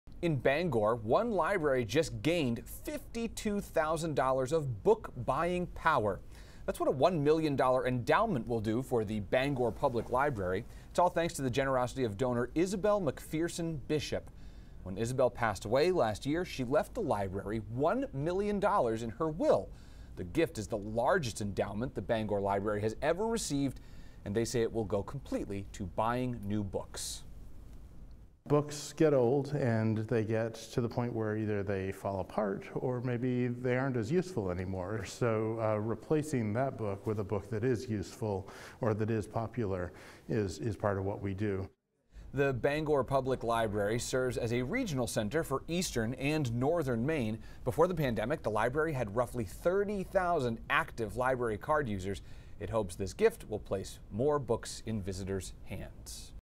Bangor Public Library receives historic donation
It's all thanks to the generosity of donor Isabel MacPherson Bishop. The gift is the largest endowment the Bangor Library has ever received.